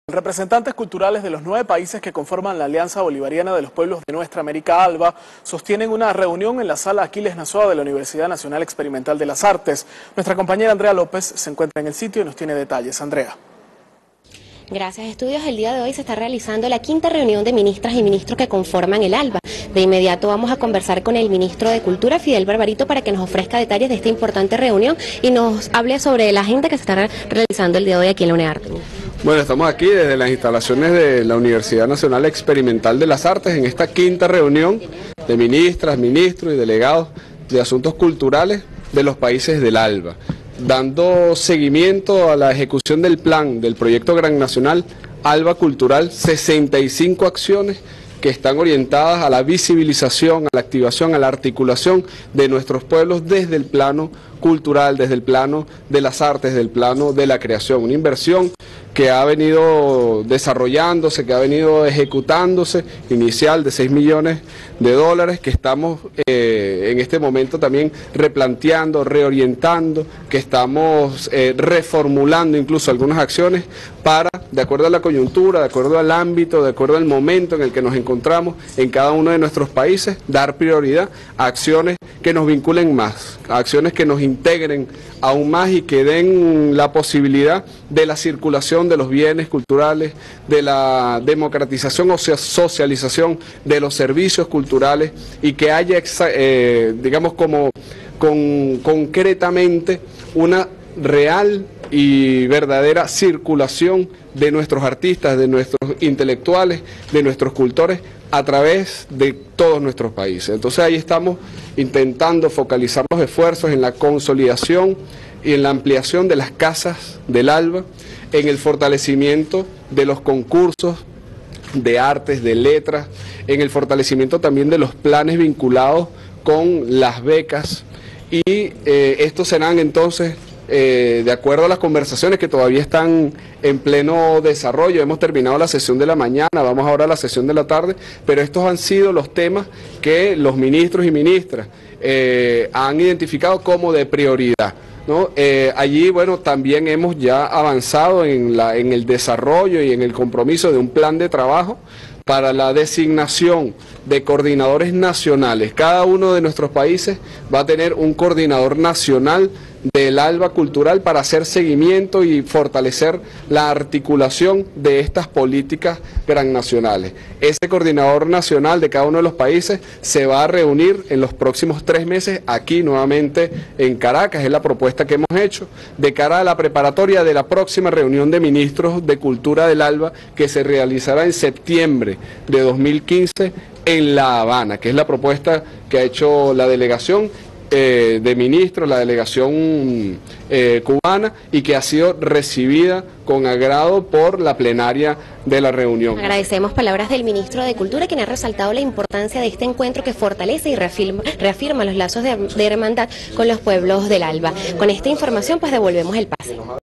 Ministro Fidel Barbarito tras reunión de ministros de Cultura del Alba
Declaraciones este 19 de agosto de 2014 desde la Unearte, en Caracas, Venezuela.